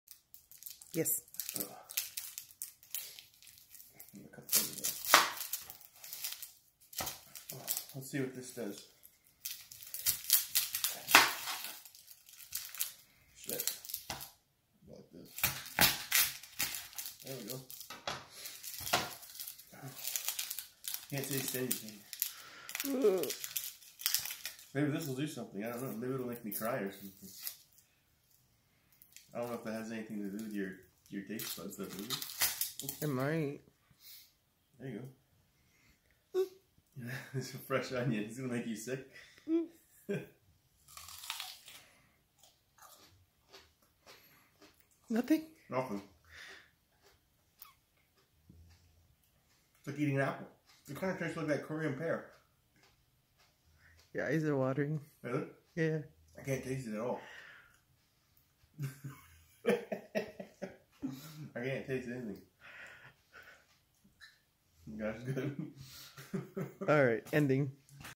No taste buds covid19
Eating a onion during covid19 recovery